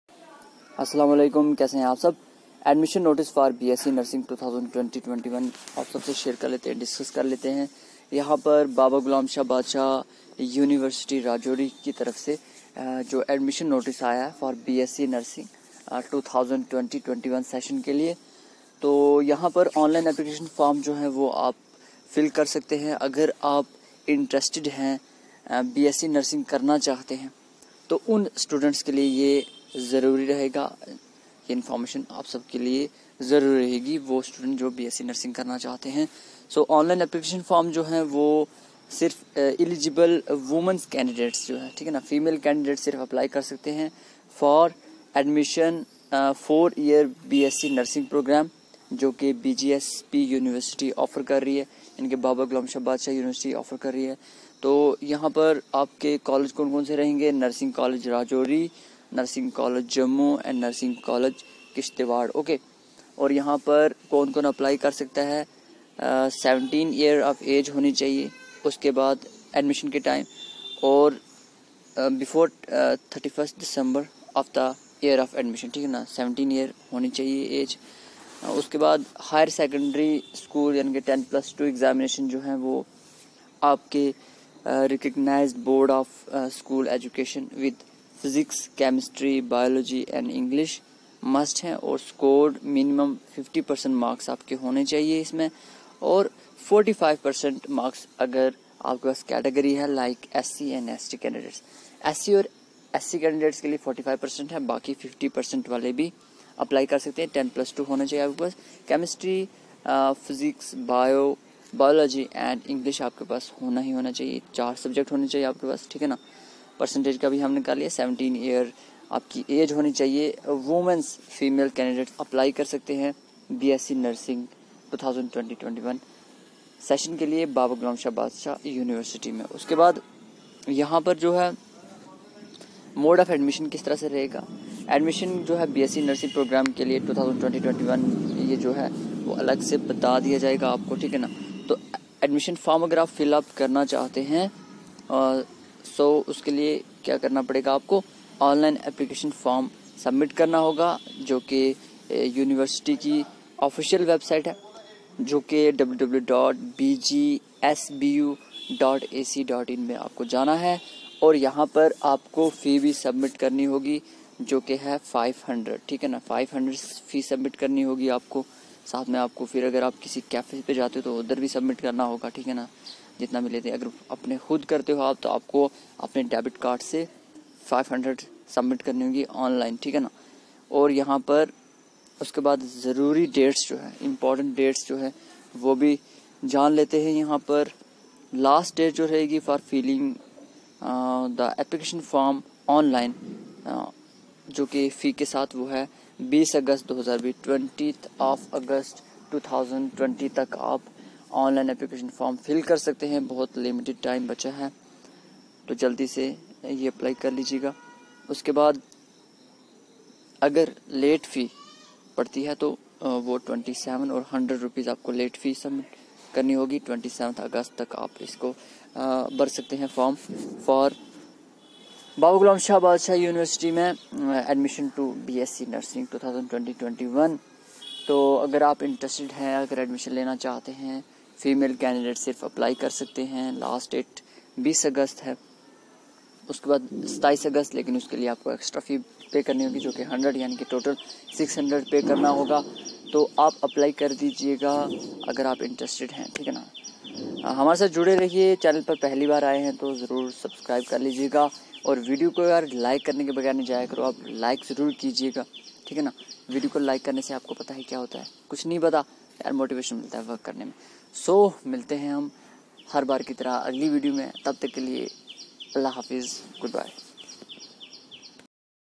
B.SC NURSING ONLINE Application form || Imo. Dates
B.sc Nursing BGSB invite online application form for the admision in Nursing Sessiin 2020-21

Inp dates For the filling up application form are 20th August last date and 27 august with late fee
Application fee is 500